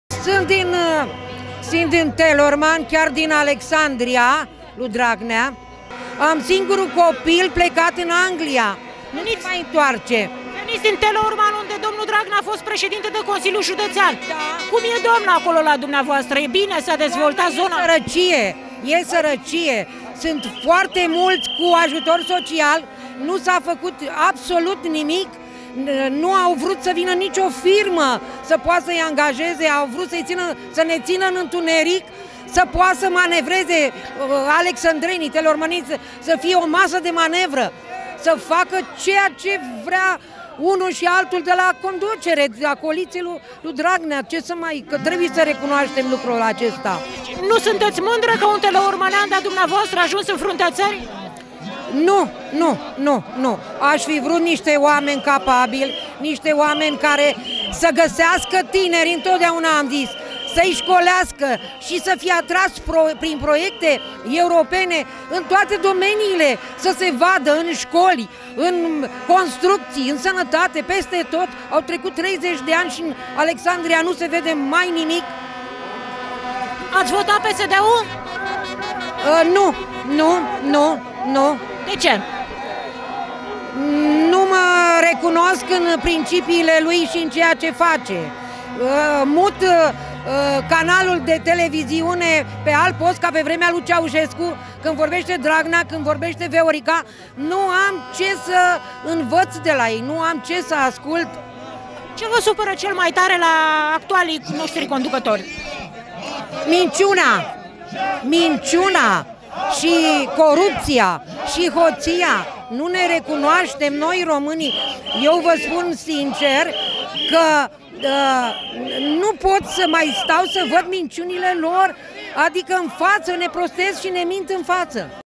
Doamnă din Teleorman la protest: Mut canalul pe alt post când vorbesc Dragnea şi Viorica
"Sunt din Teleorman, chiar din Alexandria lui Dragnea. E sărăcie la noi, foarte mulţi cu ajutor social. Nu s-a făcut absolut nimic, au vrut să ne ţină în întuneric, să fim o masă de manevră."

Reporter: Nu sunteţi mândră că un teleormănean de-al dvs. a ajuns în fruntea ţării?

"Nu, nu, nu! Aş fi vrut nişte oameni capabili. Mut canalul pe alt post când vorbesc Dragnea şi Viorica. Mă supără minciuna şi corupţia şi hoţia. Nu pot să mai stau să văd minciunile lor. Adică ne prostesc şi ne mint în faţă?!!"